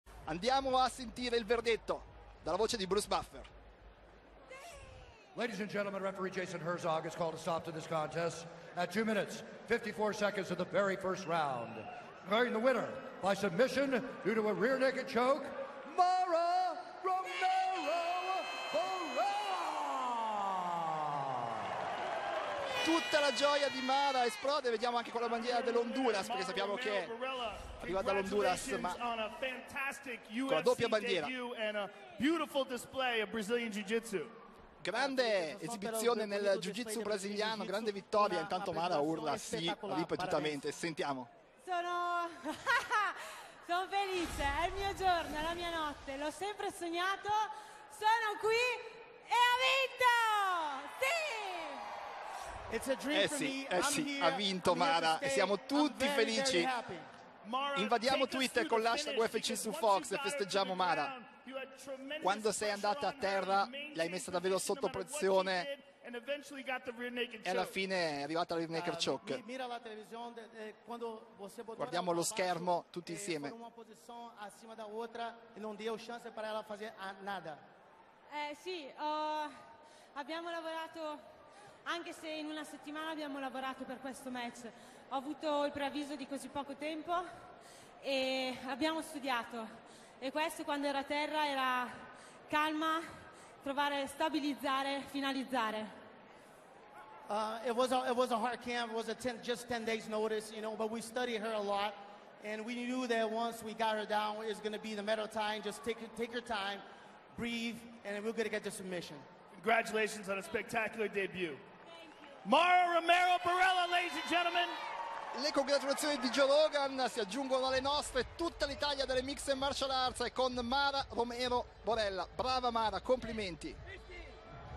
[UFC 216] Le parole di Mara Borella dopo la vittoria su Faria
Su trovi tutti i tuoi video in HD.